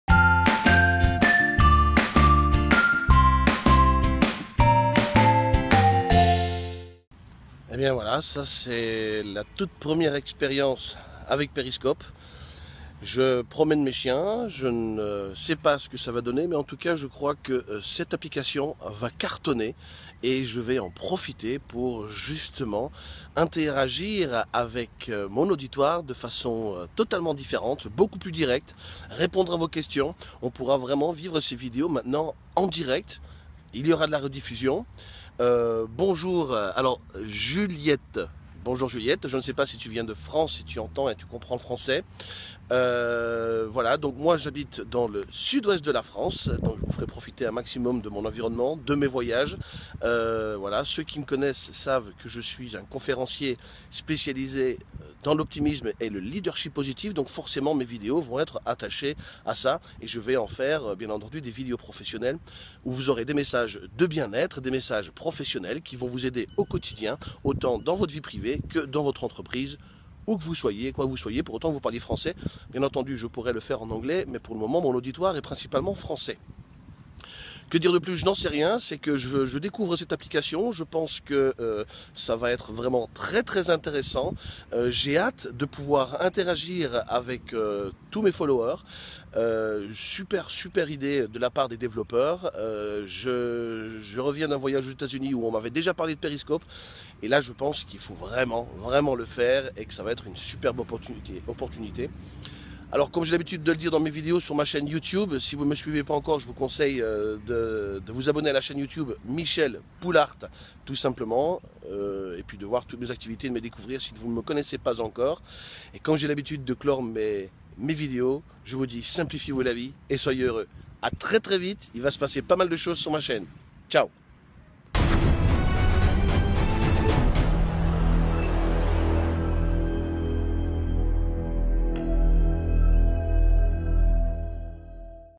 JOUR 209 : Periscope, une nouvelle dimension pour interagir en direct
L'optimisme c'est aussi accueillir les changements et les nouveautés que le monde nous proposent de façon régulière. Aujourd'hui je fais mon premier test avec l'application Periscope qui nous permettra de nous connecter en direct et d'interagir avec vous !

Periscope : MichelPoulaert